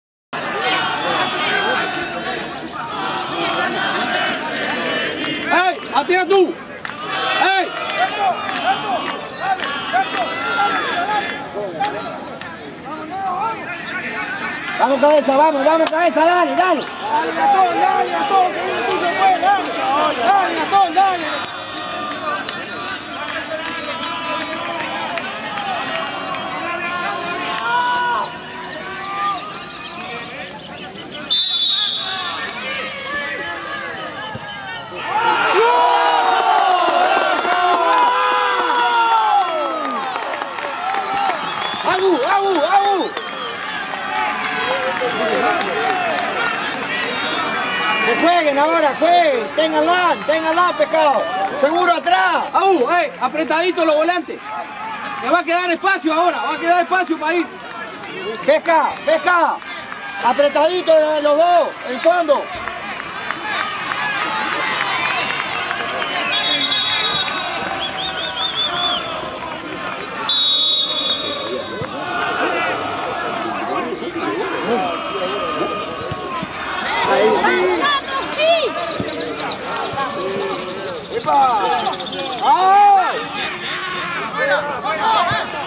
Indirecto.com.uy - La picó! - Gol de penal, Gabriel Alcoba (Ituzaingó) a Punta Ballena
Gabriél Alcoba con estupenda ejecución de tiro penal anota el 3er gol de Ituzaingó ante Punta Ballena en la victoria final 4 a 1 del equipo verde peninsular que le posibilitaría ser el campeón anticipado del Torneo Apertura 2014 de la Liga Mayor del Fútbol de Maldonado.